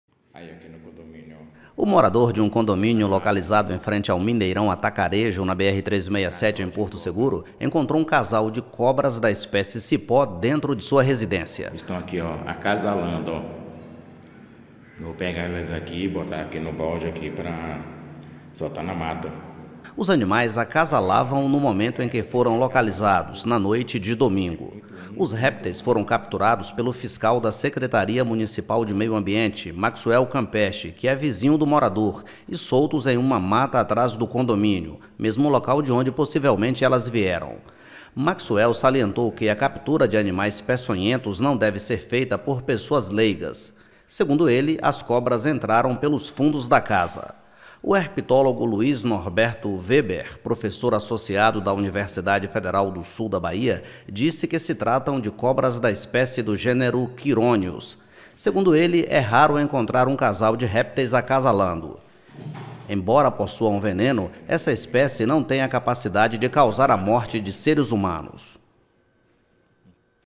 Casal de cobras cipó é encontrado acasalando dentro de residência em Porto Seguro
O morador de um condomínio localizado em frente ao Mineirão Atacarejo, na BR-367, em Porto Seguro, encontrou um casal de cobras da espécie cipó dentro de sua residência. Os animais acasalavam no momento em que foram localizados, na noite de domingo (3).